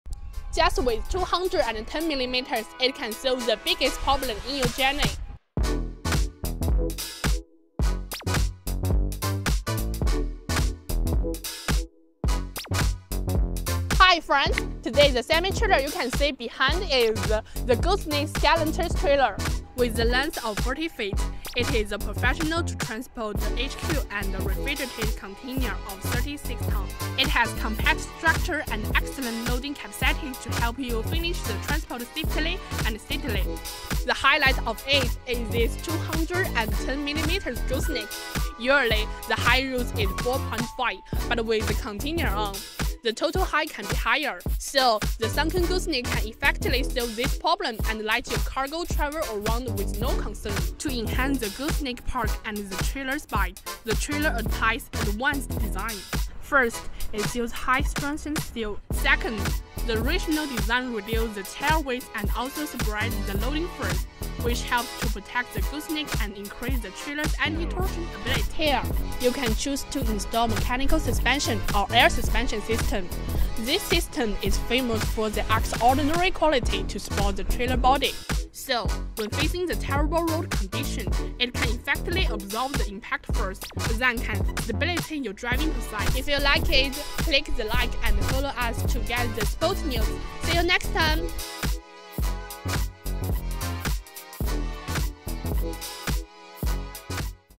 40 Feet Gooseneck Skeletal Container Chassis | Truck Chassis for Sale
Looking for a reliable container chassis to transport your cargo? Check out our 40 feet gooseneck skeletal container chassis, designed to provide exceptional stability and ease of use. This standard skeleton trailer is perfect for hauling 40ft containers with its 3-axle configuration. Whether you're in the shipping industry or need a dependable solution for moving goods, this gooseneck trailer is built to handle heavy loads with efficiency and durability.

In this video, we explore the features and benefits of our 40ft gooseneck skeletal container chassis. We'll discuss its sturdy truck chassis, designed to withstand the rigors of long-haul transportation. You'll also discover the competitive pricing options available for this 3-axle skeletal trailer, allowing you to make an informed decision for your business needs.

Don't miss out on this opportunity to invest in a reliable container chassis for your transportation requirements. Watch the video to learn more about the 40 feet gooseneck skeletal container chassis and find out how it can enhance your cargo hauling capabilities. Get in touch with us today to inquire about the 40' 3-axle skeletal trailer price and take your business to the next level.